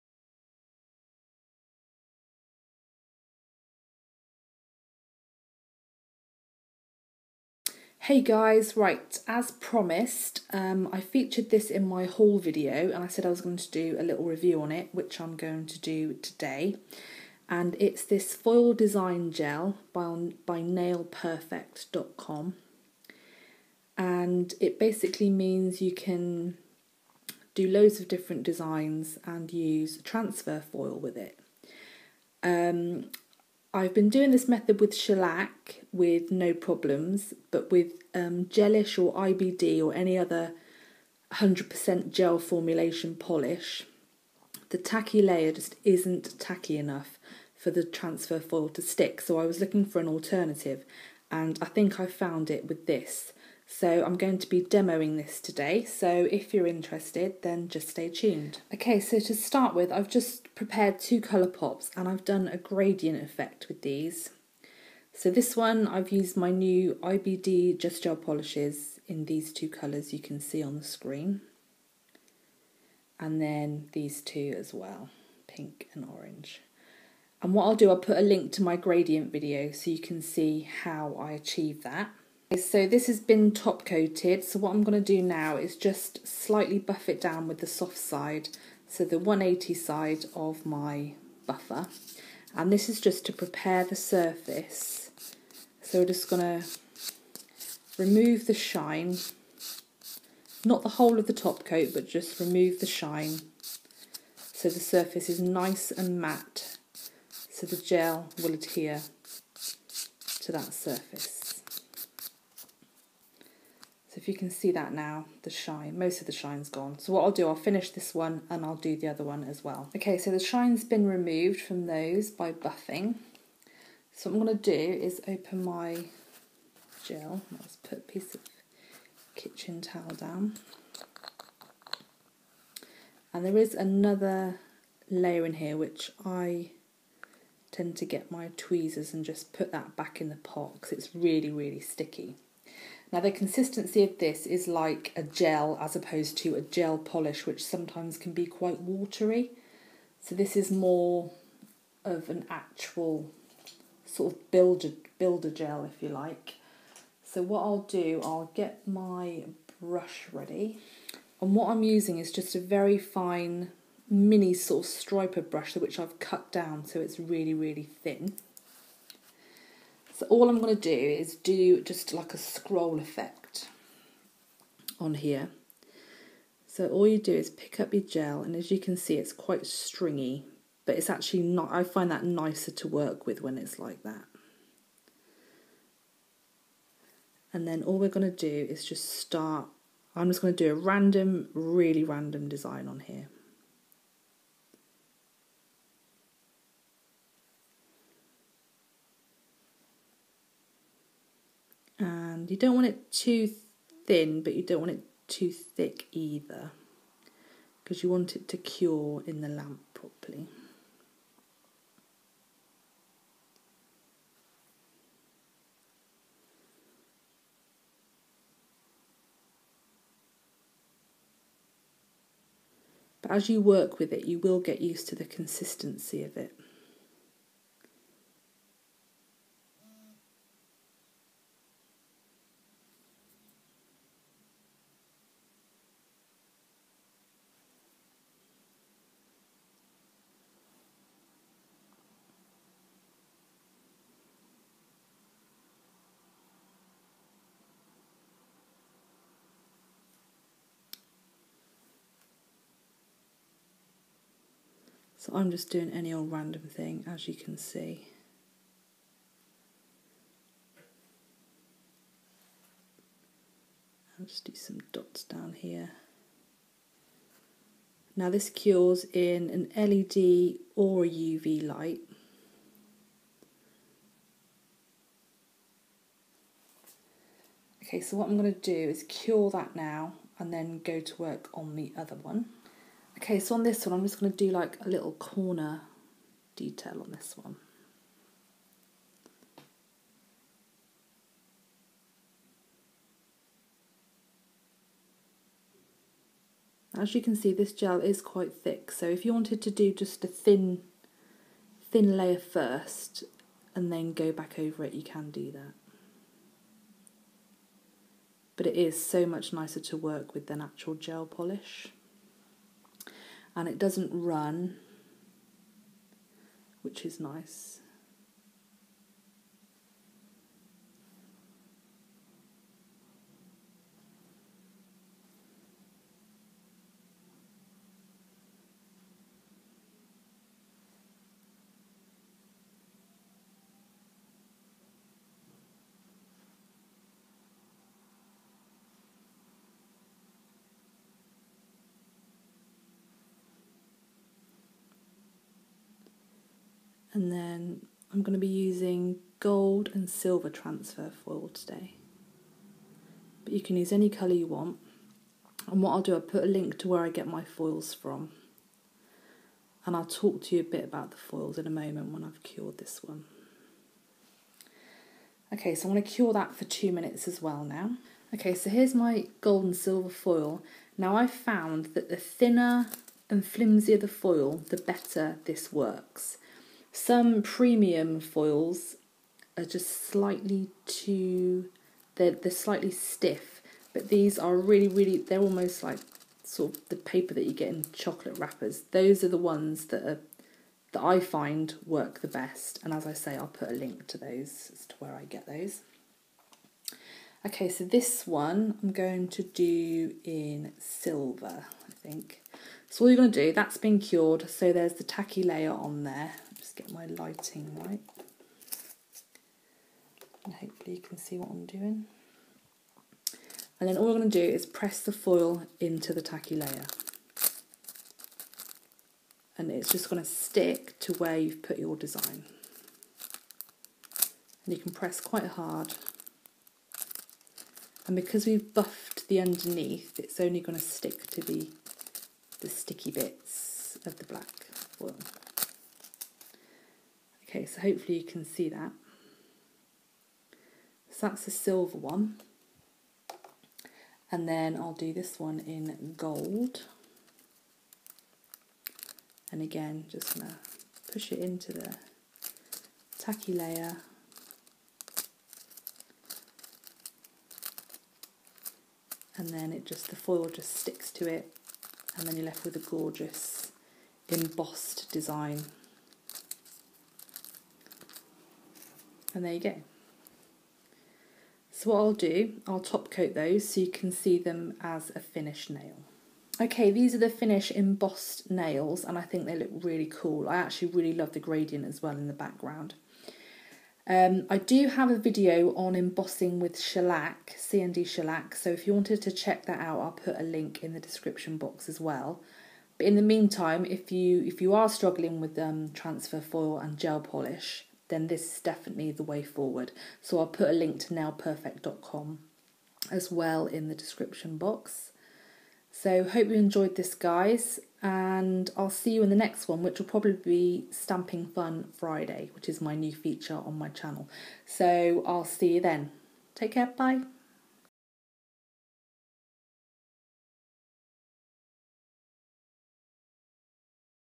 Review: Nailperfect.net Foil Design Gel for embossing nail art
Review of this great foil design gel by Nailperfect.com or Nailperfect.net depending on where you

Sarah xoxo